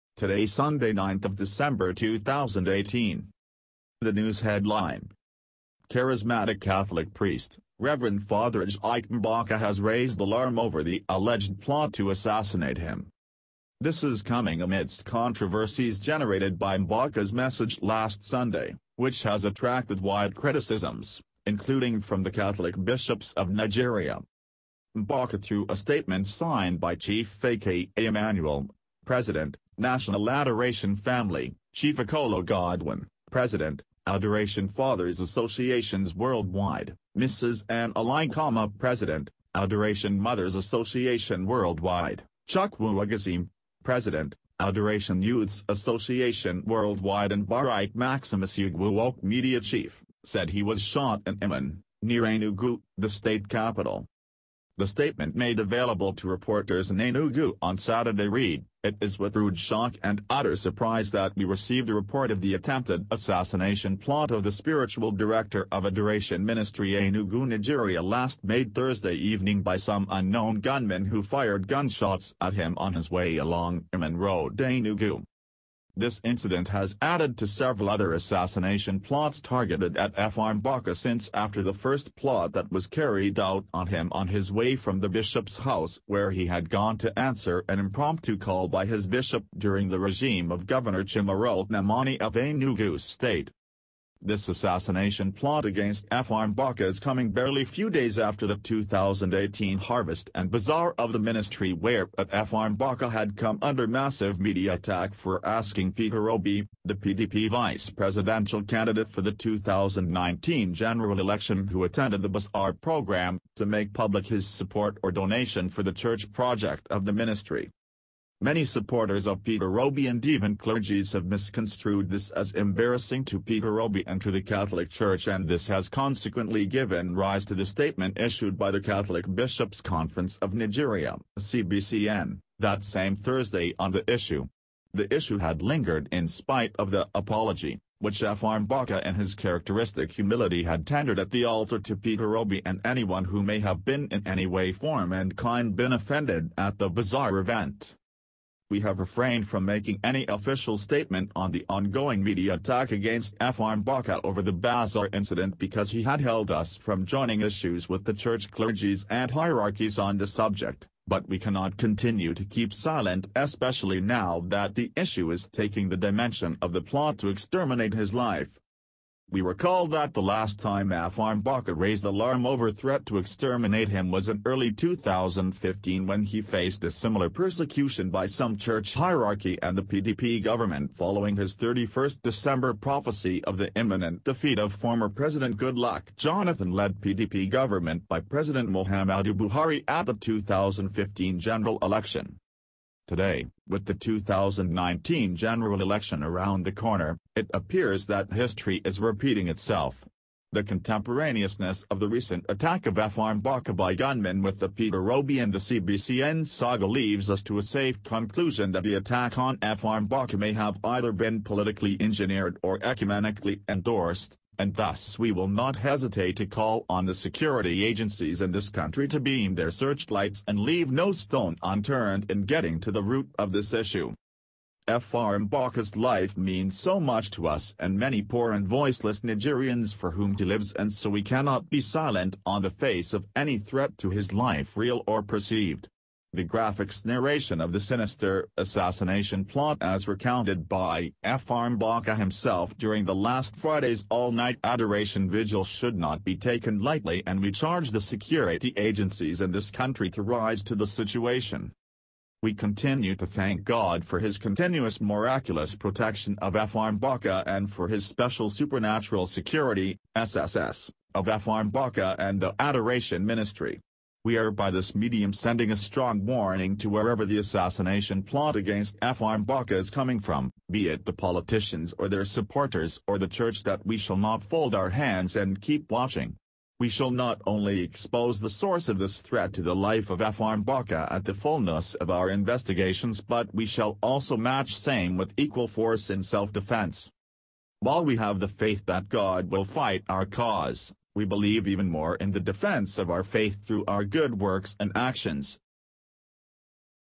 NNAMDI KANU: RF MBAKA ALLEGED PLOT TO ASSASSINATE HIM..
RF MBAKA ALLEGED TO BE ASSASSINATED FOLLOWING THE CRITICIZED MEETING HE HAD WITH THE FORMER GOVERNOR OF ABIA STATE, PETER OBI.